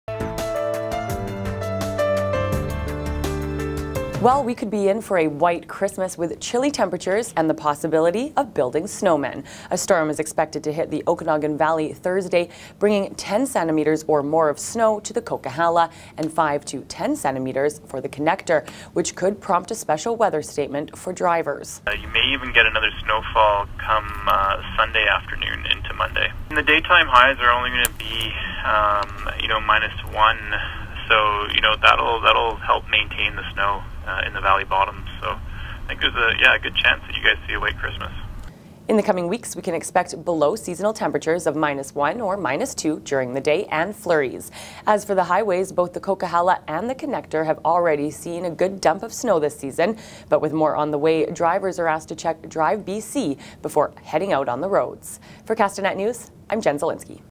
Snow for Christmas
News Brief with Jen Zielinski: The Okanagan might be in for a white Christmas.